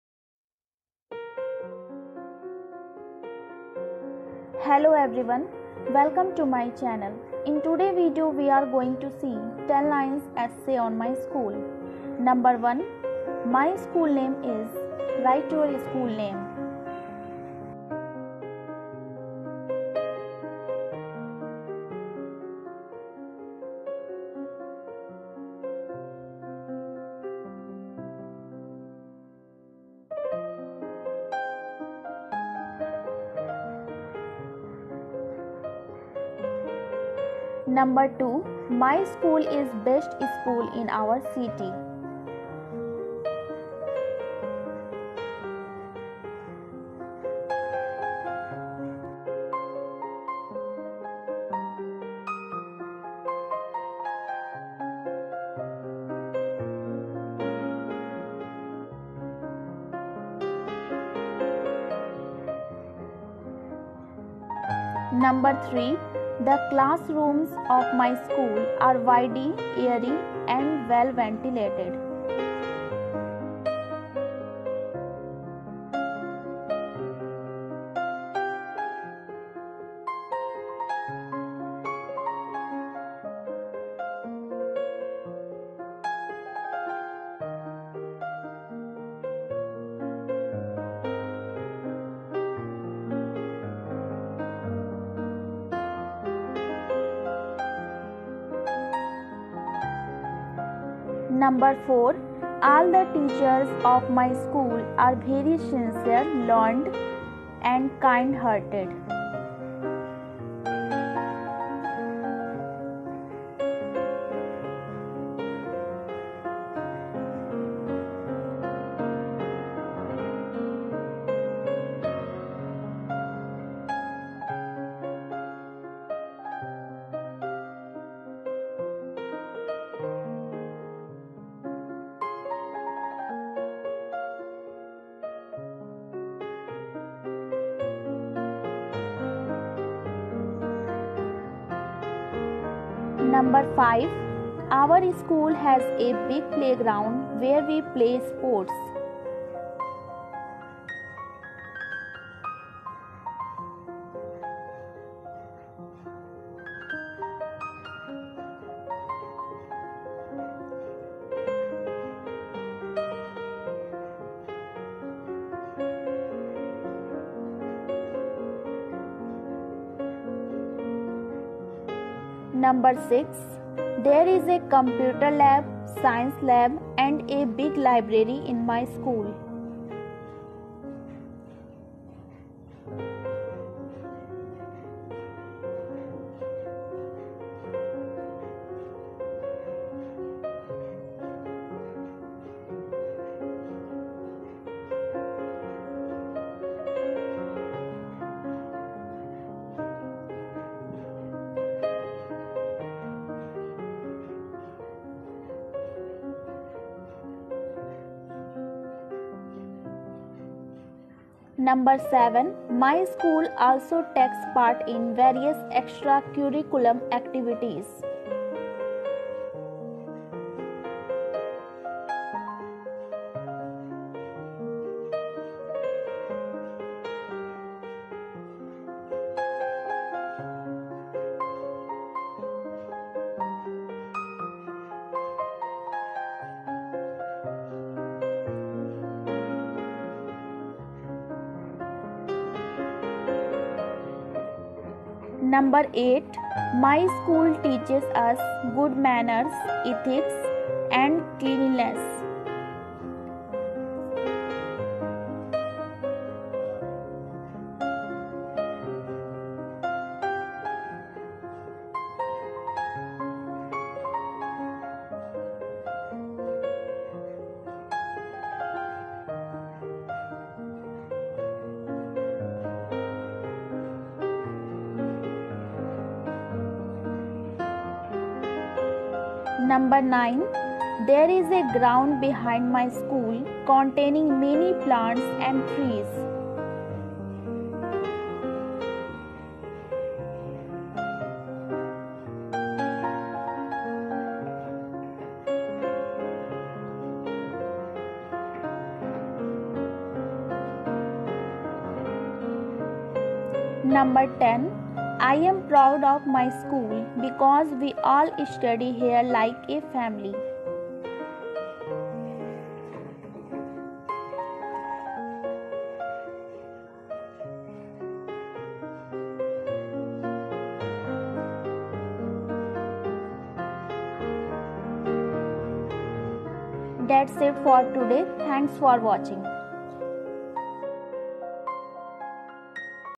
My school-10 lines essay in english
In this video you will see 10 lines essay on my school.

      Thankyou😊